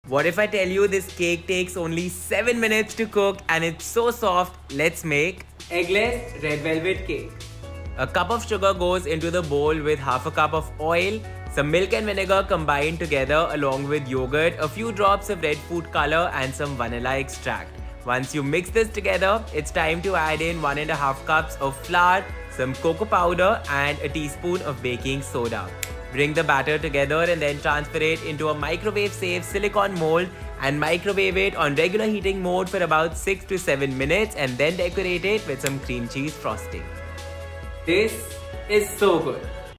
*7 MINUTE* EGGLESS RED VELVET CAKE RECIPE | HOW TO MAKE CAKE IN A MICROWAVE
Looking for a cake recipe that gets ready in less than 10 minutes in a microwave💁🏻‍♂️ ? Then this eggless Red Velvet cake is going to top your list ❤️With a super soft texture and an incredible red velvet flavour this recipe will make for an indulgent weekend treat, my word 💯🙌🏻

Ingredients
✨1 cup castor sugar
✨1/2 cup oil
✨1 + 1/4 cup buttermilk
✨5-6 drops of red food colour

Method
1. In a bowl, add sugar, oil, buttermilk, yogurt, vanilla and red colour and whisk till everything is well combined.
2. Sift in the all purpose flour, cocoa powder and baking soda and mix well till you get a smooth batter.
3. ⁠Pour the batter into the silicon mould and cook it in the microwave for 7 minutes at normal cooking mode.
4. ⁠Once cooked, take it out of the microwave and let it cool down.
6. In a bowl add in softened butter and softened cream cheese. Using an electric whisk, whip the two together for 2 minutes.
7. Add in vanilla and whip again for a minute.
8. Sift in icing sugar and whip everything for another 2 minutes or until you see the frosting hold shape.
9. Finish the cake by spreading a generous layer of cream cheese frosting on top and garnishing with some cake crumbs.